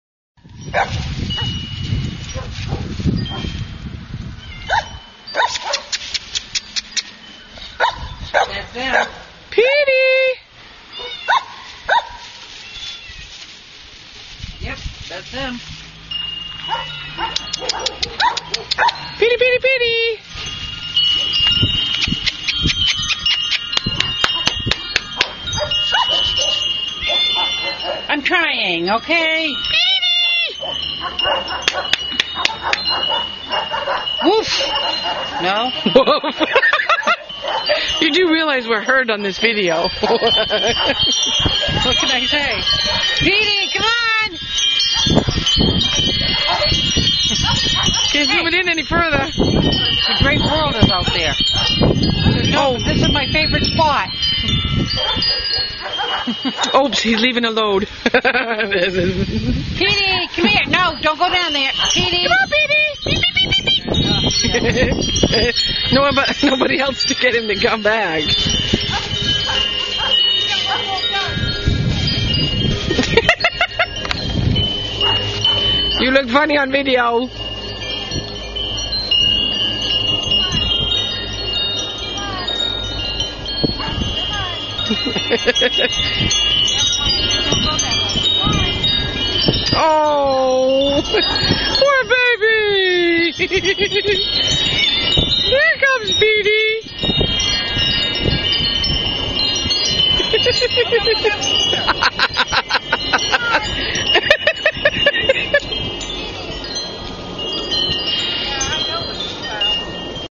pistol pete
Video of a cute Chinese Shar Pei puppy for sale. pistol pete